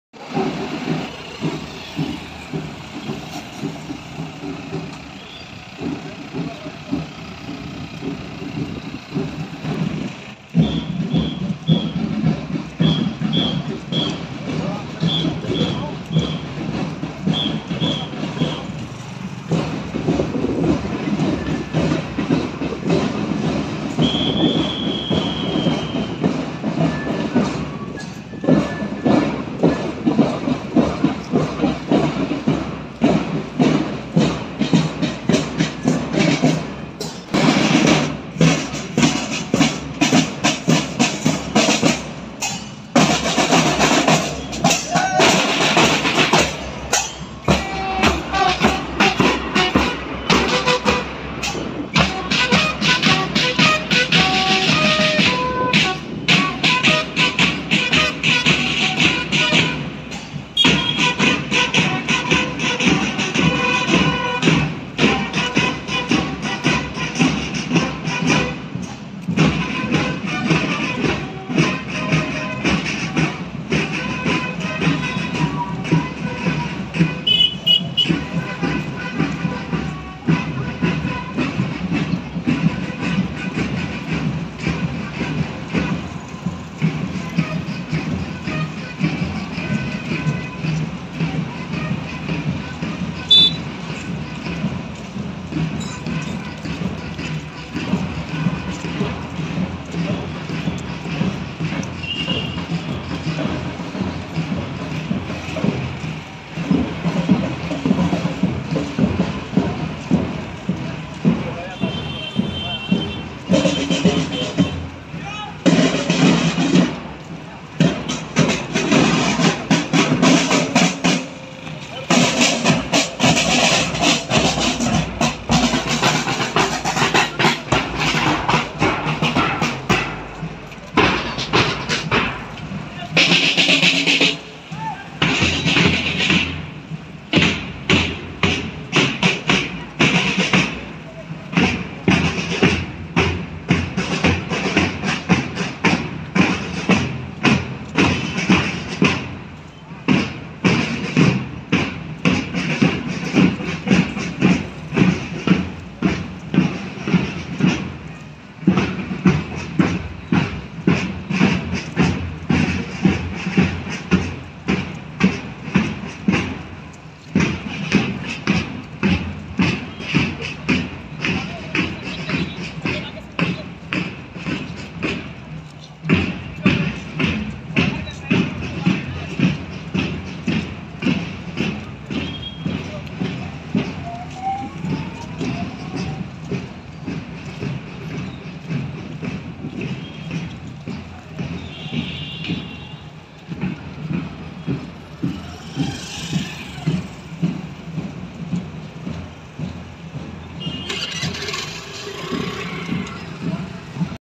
RSS chief Mohan Bhagwat visited with RSS karyakarta in Ambikapur (C.G.)
RSS rally in Ambikapur , Mohan Bhagwat visited to Ambikapur.